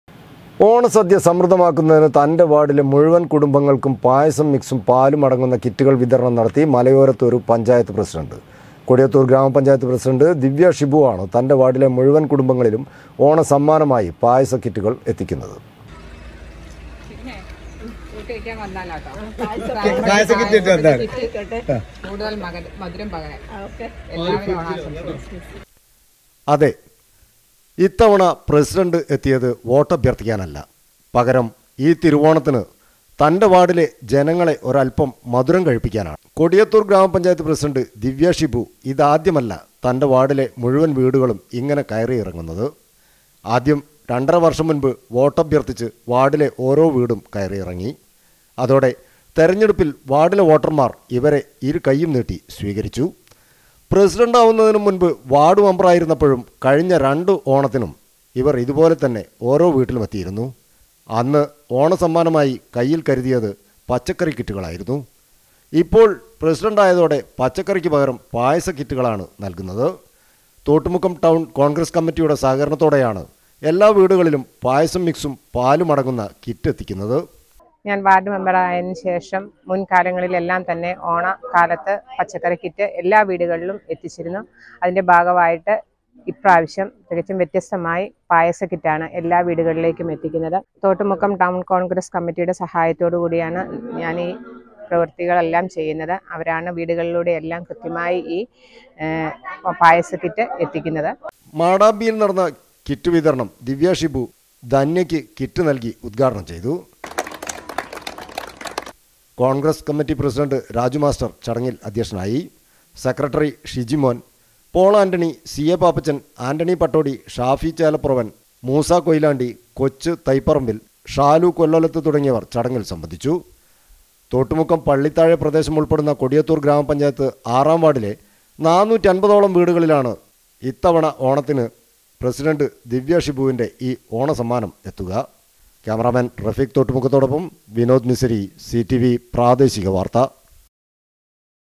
പായസം മിക്‌സും, പാലും അടങ്ങുന്ന കിറ്റുകൾ വിതരണം നടത്തി മലയോരത്ത് ഒരു പഞ്ചായത്ത് പ്രസിഡന്റ്.
ഓണസദ്യ സമൃദ്ധമാക്കുന്നതിനു തന്റെ വാർഡിലെ മുഴുവൻ കുടുംബങ്ങൾക്കും പായസം മിക്‌സും, പാലും അടങ്ങുന്ന കിറ്റുകൾ വിതരണം നടത്തി മലയോരത്ത് ഒരു പഞ്ചായത്ത് പ്രസിഡന്റ്.